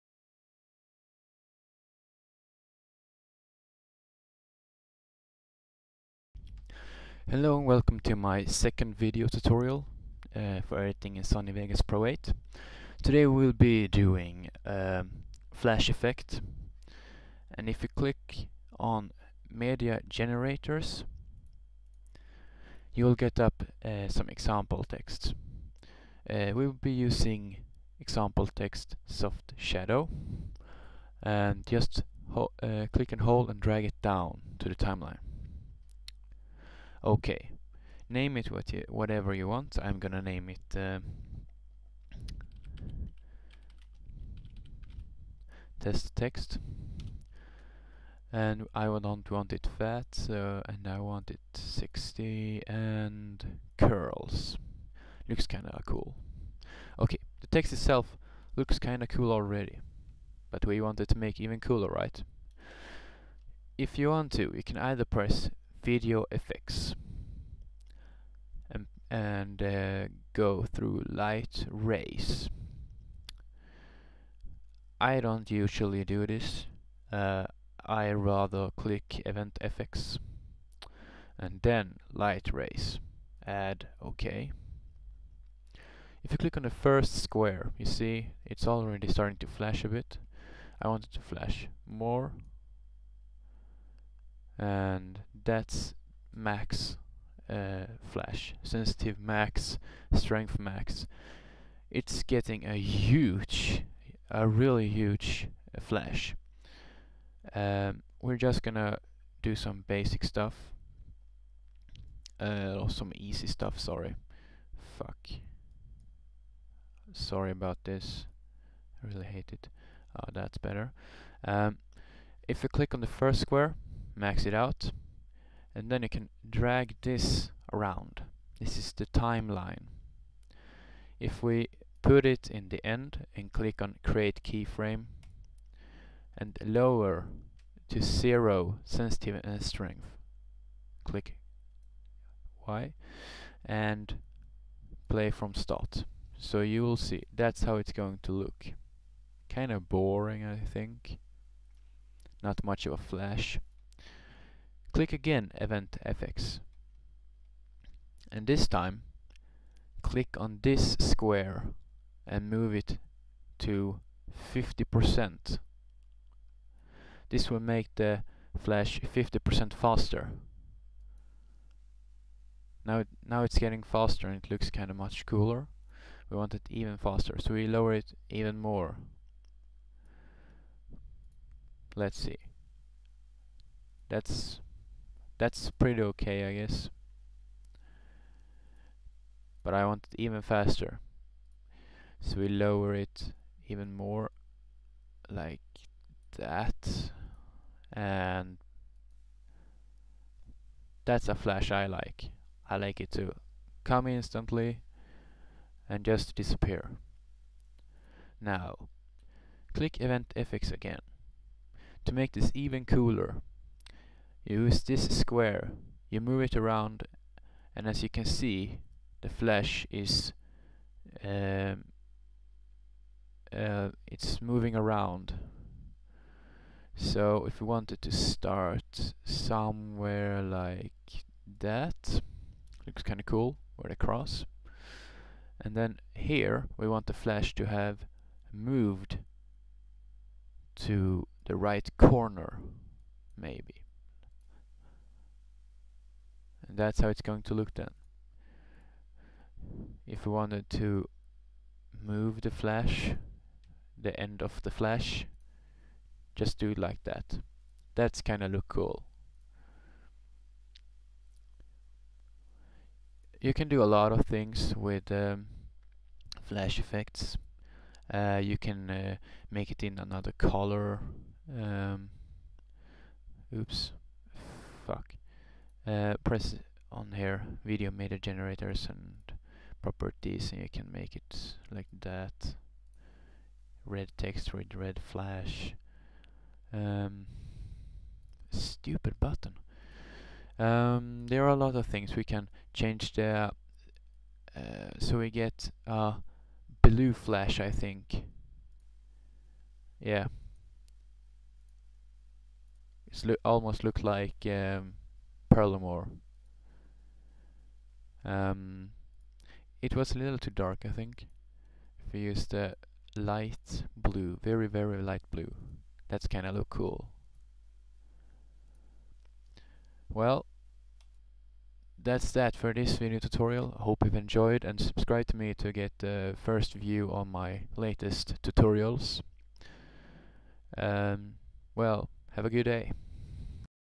2nd Video Tutorial of Sony Vegas PRO 8.0 (Light Rays)
here is a video to show you how tyo make flash effects in Sony Vegas. It was requested almost instantly after my first release on tutorials in Sony Vegas
Hope that it will help some of you guys/girls to make flash effects in Vegas.